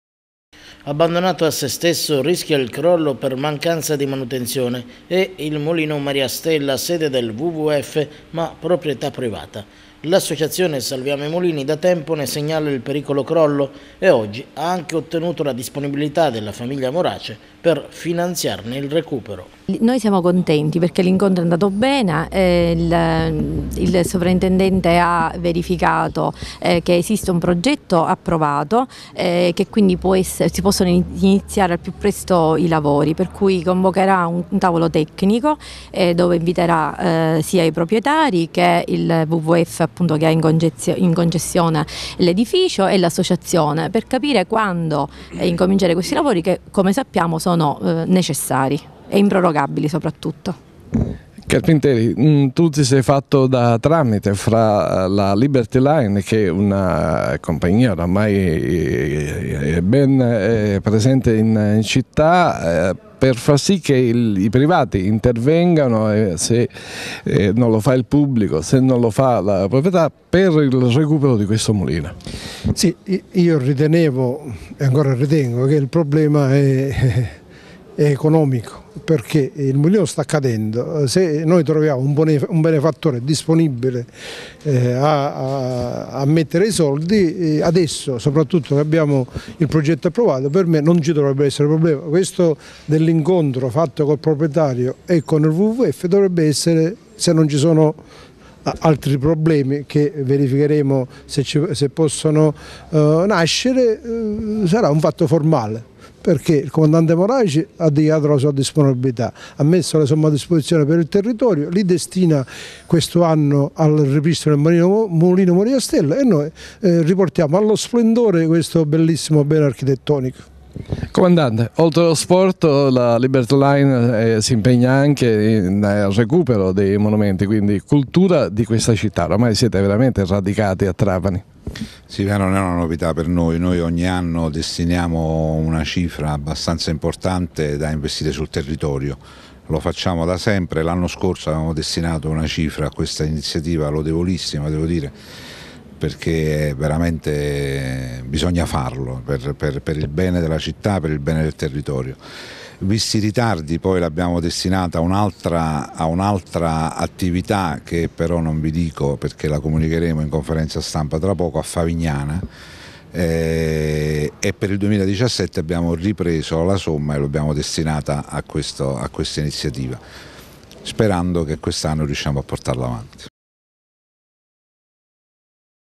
I Morace salvano il mulino
La famiglia Morace che finanzierà la ristrutturazione del mulino Maria Stella, da mesi a rischio di crollo.